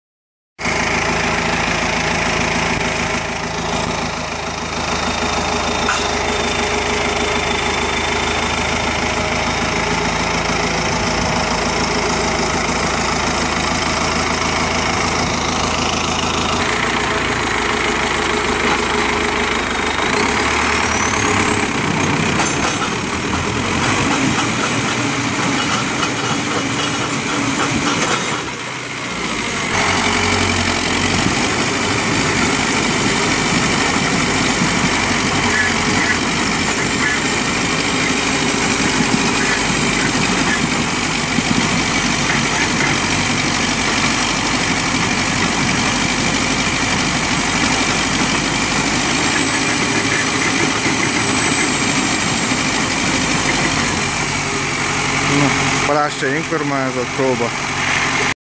MTZ 920.3 traktor kerékhajtás és váltó tesztelése
Az érintett MTZ 920.3-as traktorról a tulajdonosa azt állította, hogy terhelés alatt a váltót (terep) kidobja üresbe. Illetve állítólag a motorja feketén füstölve jár. Hát mi ilyet nem tapasztaltunk. De próba jól sikerült. :)